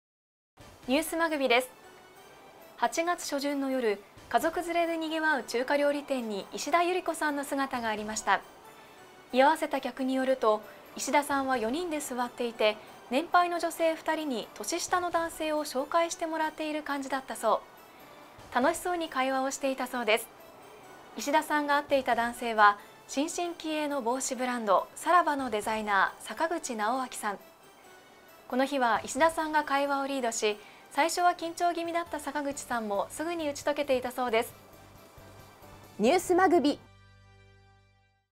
石田ゆり子 、年下イケメン デザイナー と ワンピ で 中華な夜
News MagViです。

8月初旬の夜、家族連れで賑わう中華料理店に石田ゆり子さんの姿がありました。

居合わせた客によると石田さんは4人で座っていて、年配の女性2人に年下の男性を紹介してもらっている感じだったそう。

楽しそうに会話をしていたそうです。

石田さんが会っていた男性は、新進気鋭の帽子ブランド『サラヴァ』のデザイナー・坂口直顕さん。

この日は、石田さんが会話をリードし、最初は緊張気味だった坂口さんも、すぐに打ち解けていたそうです。

◆石田ゆり子、年下イケメンデザイナーとワンピで中華な夜

アナウンサー：藤原優紀